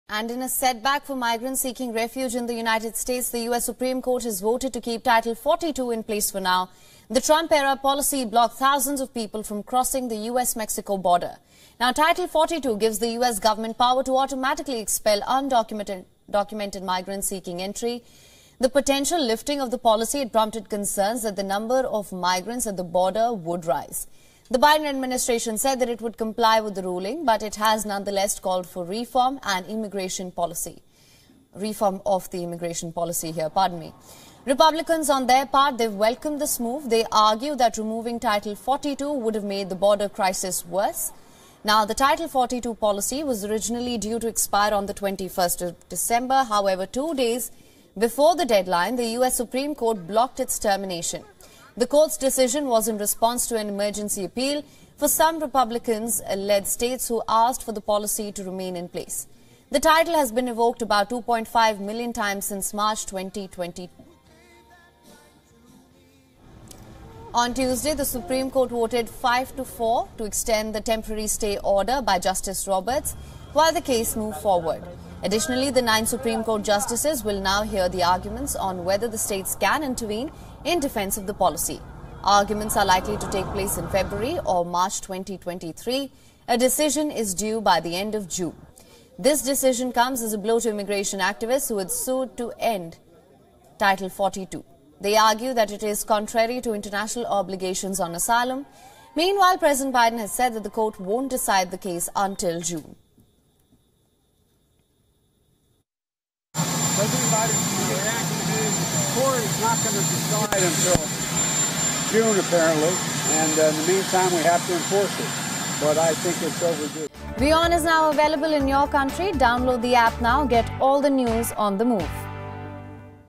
US: Trump-era migrant policy to stay for now; top court rules to keep Title 42 in place | WION
In a setback for migrants seeking refuge in the United States, the US Supreme Court has voted to keep Title 42 in place. The Trump-era policy blocked thousands of people from crossing the US-Mexico border. Title 42 gives the US government power automatically expel undocumented migrants seeking entry inside the country.